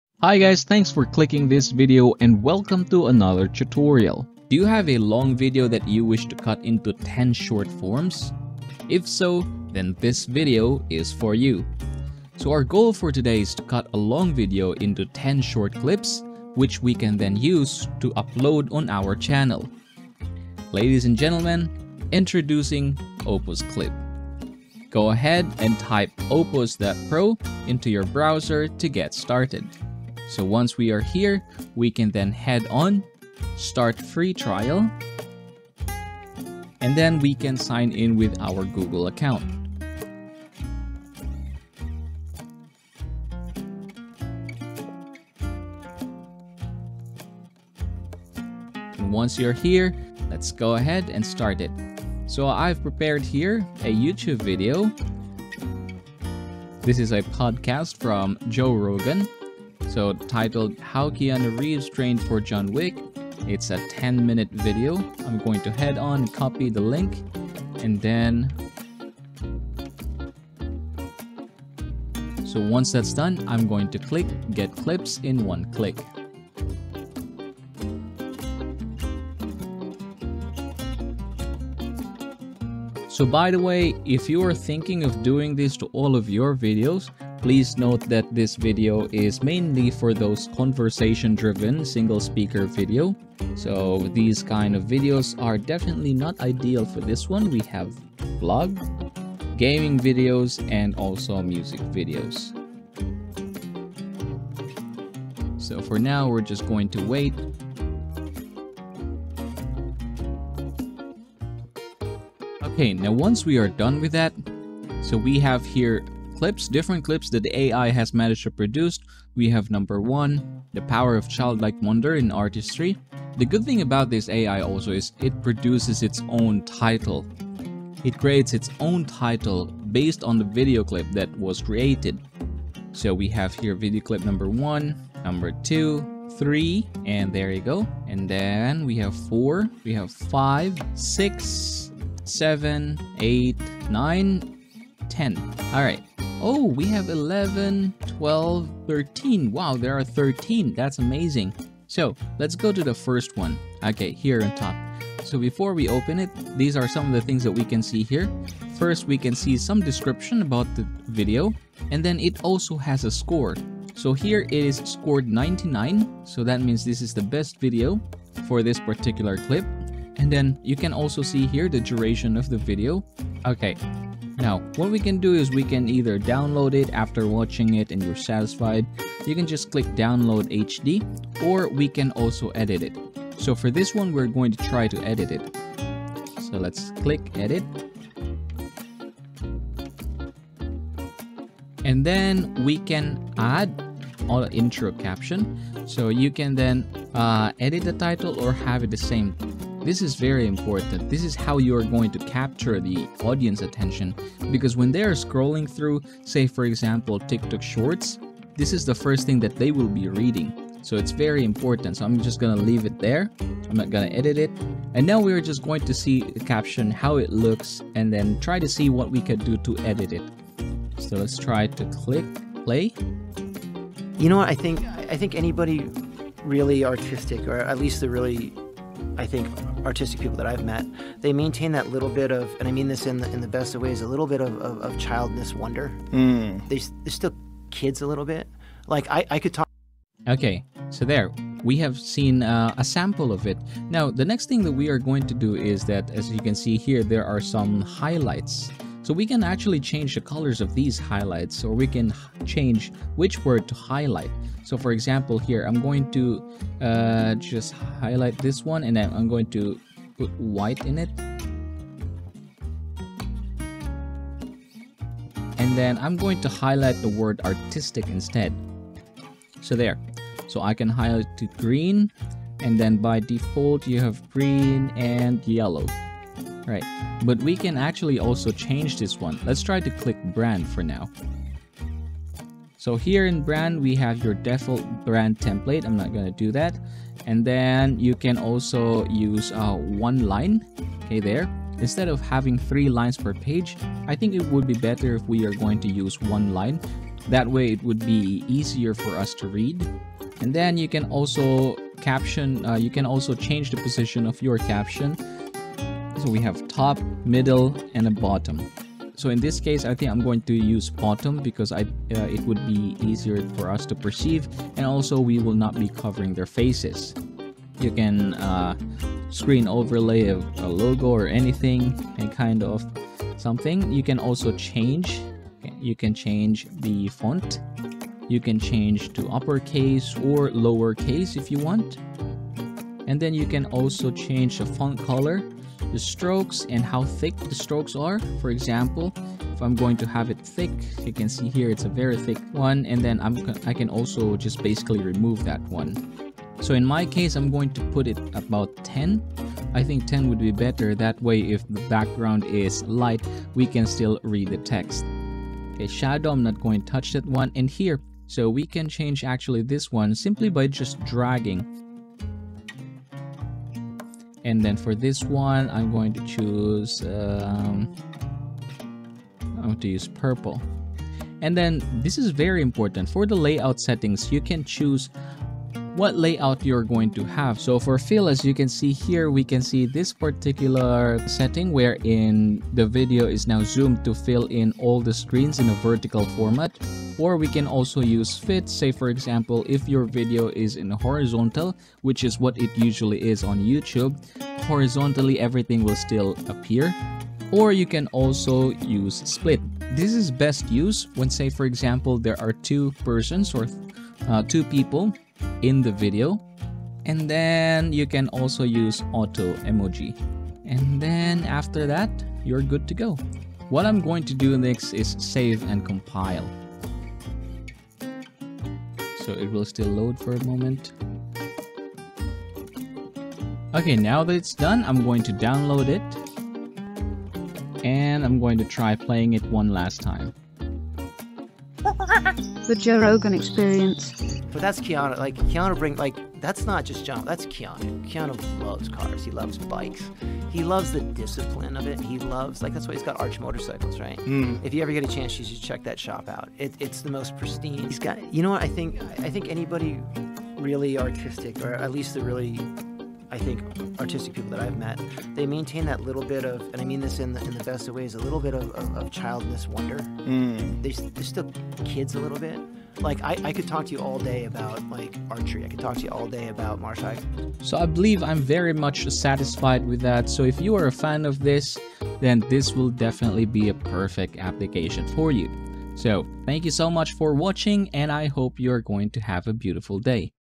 How To Cut Long Video Into Shorts Using AI Within 1 Minute (Beginner Tutorial)
Welcome to this tutorial on how to cut long videos into shorts using Opus Clip, an advanced AI-powered video editing tool. If you've ever wanted to transform lengthy videos into bite-sized, engaging clips, you're in the right place. In this step-by-step guide, we'll walk you through the process of utilizing Opus Clip to seamlessly cut and edit long videos into captivating shorts.

With Opus Clip, you can easily repurpose your content for social media platforms and create visually stunning short clips that grab viewers' attention. We'll start by introducing you to Opus Clip and guiding you through the setup process.

Once you're set up, we'll demonstrate how to upload your long video and effortlessly generate multiple short clips with just a few clicks. Opus Clip's advanced AI algorithms will automatically analyze and segment your video, providing you with a collection of professionally edited clips. You'll also have the option to customize titles and captions to make each clip more impactful and engaging.

Throughout the tutorial, we'll provide tips and insights on optimizing your short clips for different platforms, including TikTok and other social media channels. Learn how to add captivating intros, customize highlight colors, fonts, and layout settings, and create a cohesive visual style that aligns with your brand.

By the end of this tutorial, you'll have a collection of attention-grabbing short clips ready to be shared across various platforms. Join us on this Opus Clip tutorial journey and discover how to cut long videos into shorts like a pro. Start captivating your audience with compelling, bite-sized content today.